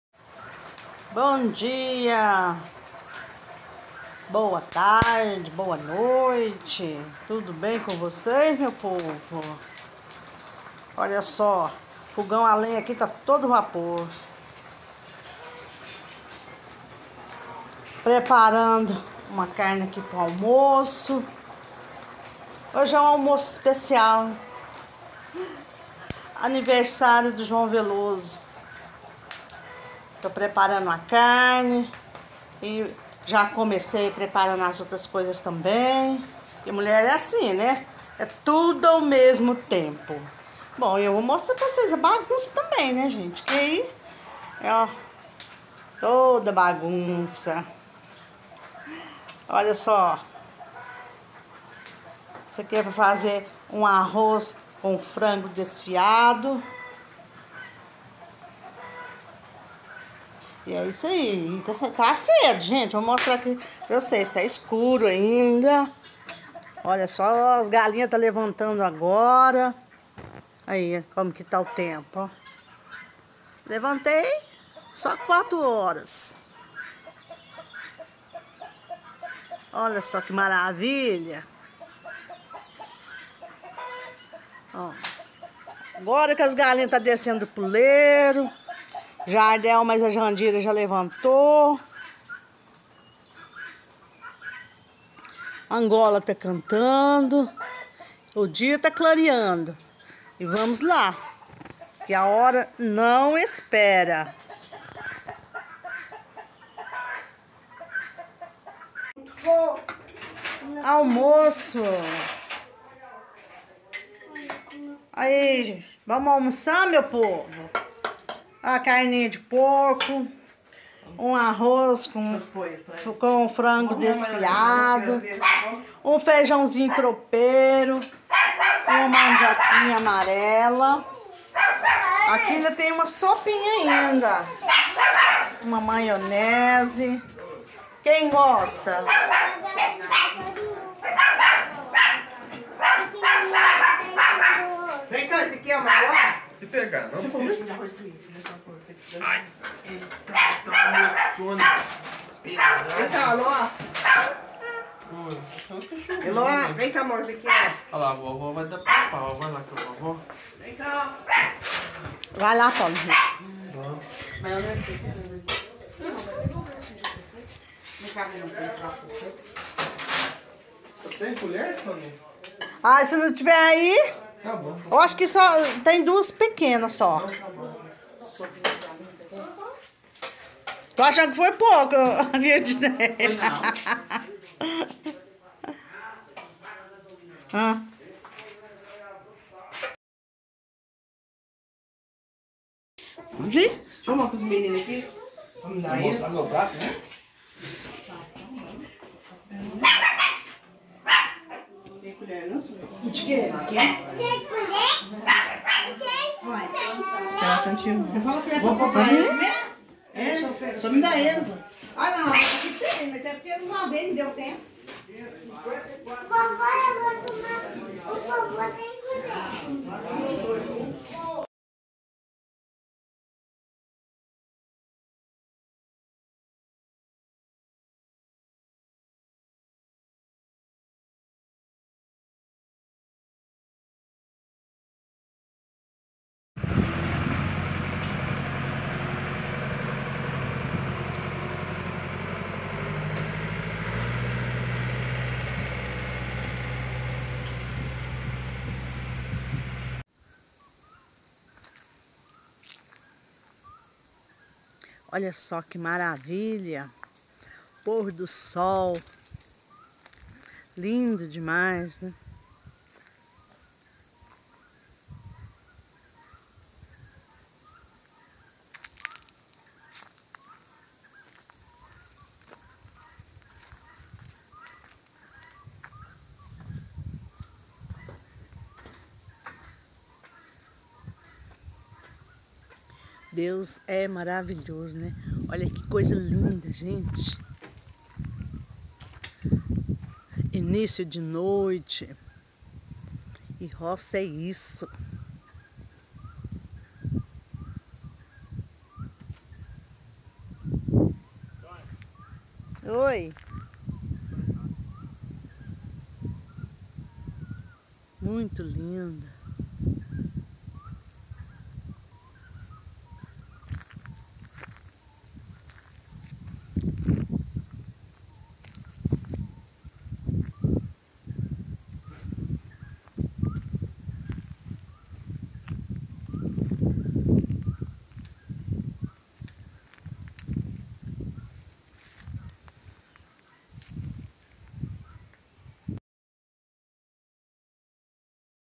ALMOÇO NO FOGÃO A LENHA 🪵 🎂 ANIVERSÁRIO DO JOÃO VELOSO!!
filmora filmoramobile. Boa tarde meu povo, tudo bem com vocês?
Comigo tudo bem graças a Deus. Fiz um almoço simples para comemorar o aniversário 🎂 do João Veloso. Gente ficou uma delícia 😋 esse almoço.

Endereço para envios e correspondência.Caixa postal 17 CEP 38840970 Carmo do Paranaíba MG.